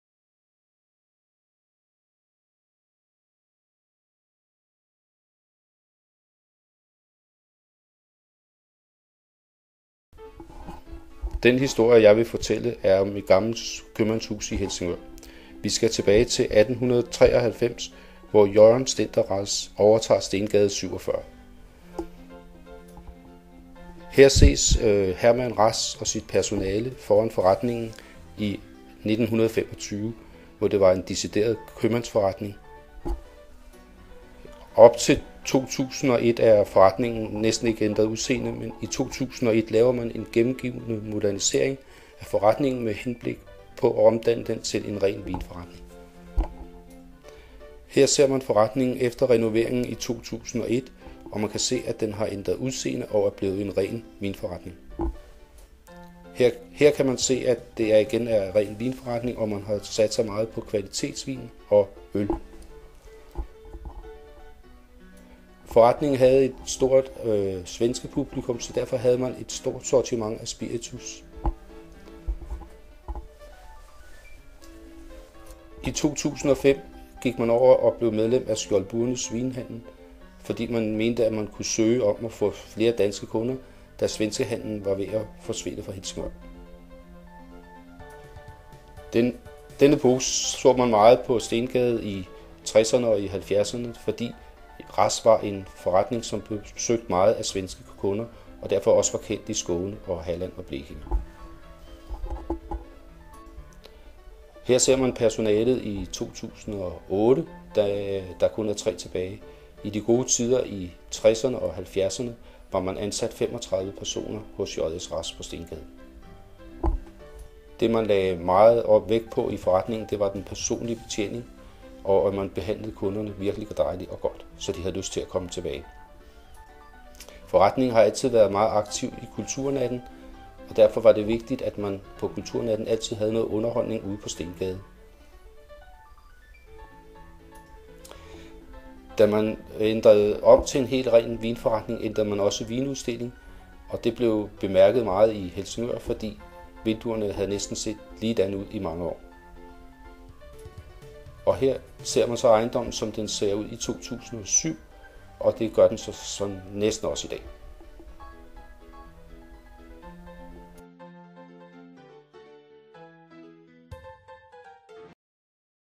Rasch en købmandsbutik fra Sundtoldtiden
En fortælling om et købmandshus på Stengade i Helsingør fra 1893 til årtusindskiftet.
Fortalt af Preben Frederiksen.